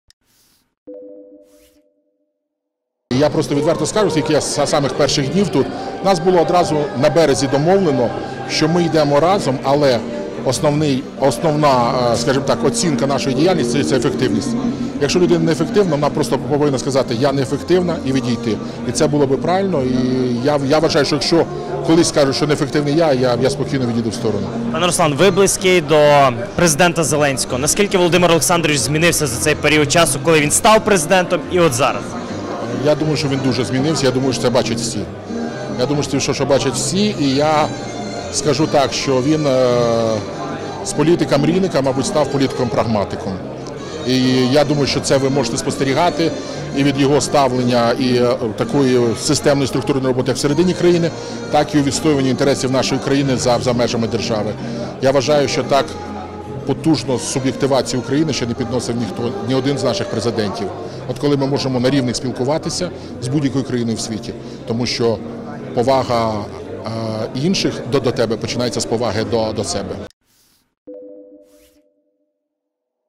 ЗЕЛЕНСЬКИЙ вже не політик-мрійник, тепер він політик-прагматик | Руслан Стефанчук
«Зеленський вже не політик-мрійник, тепер він політик-прагматик більше», - перший заступник голови Верховної ради Руслан Стефанчук